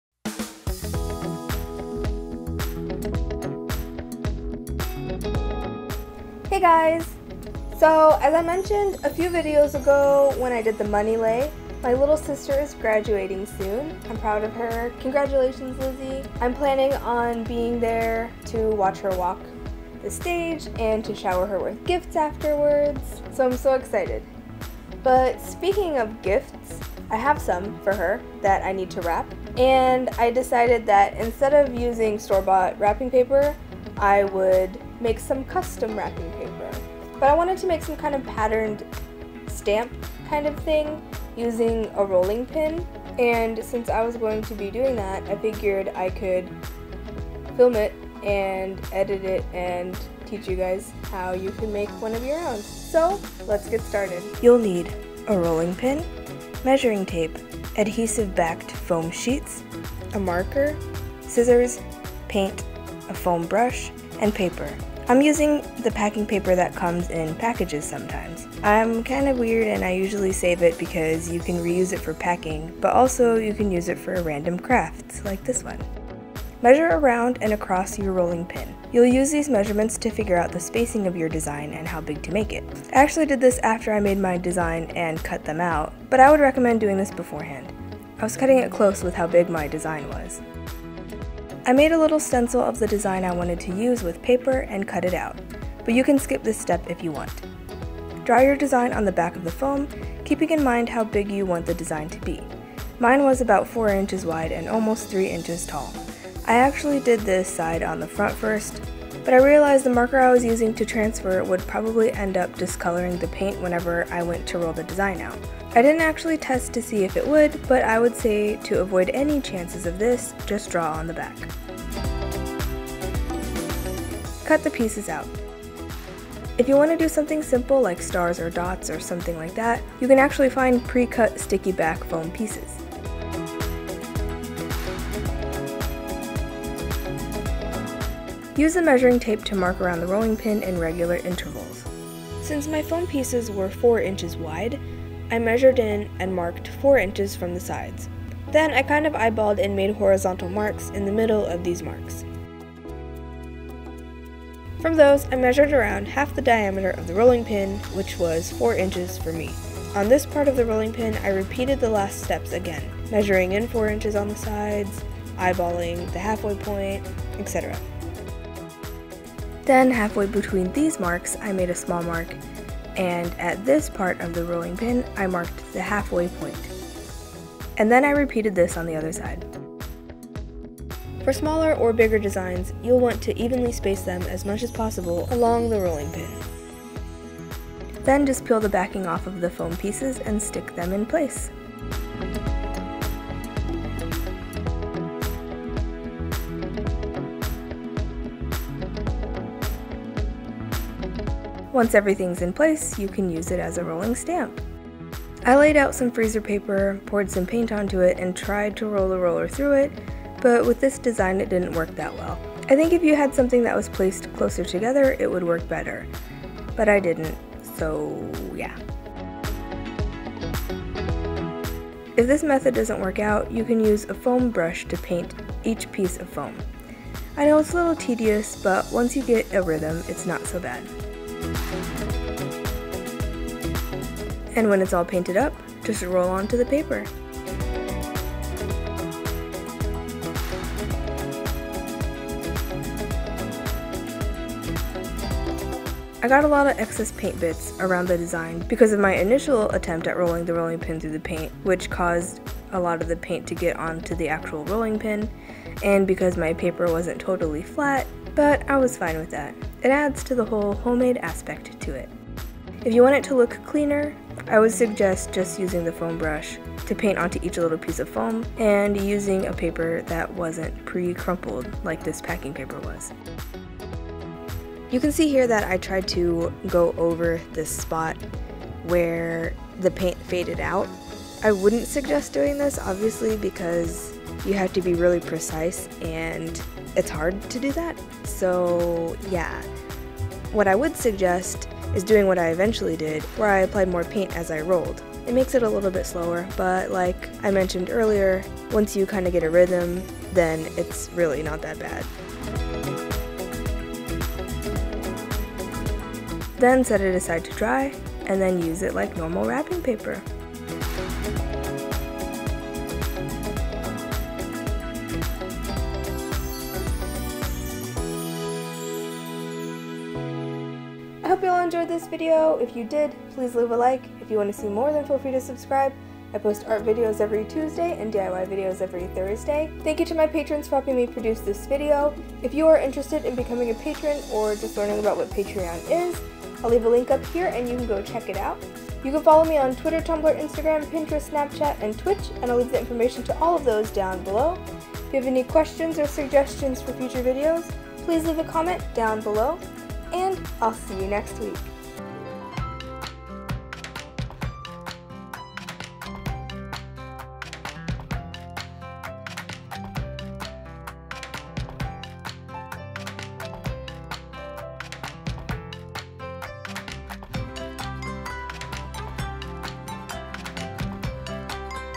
How to Make Stamped Customized Gift Wrap with Rolling Stamp
･ﾟ✧◃SUPPORT ME! ▷

･ﾟ✧◃SOCIAL MEDIA! ▷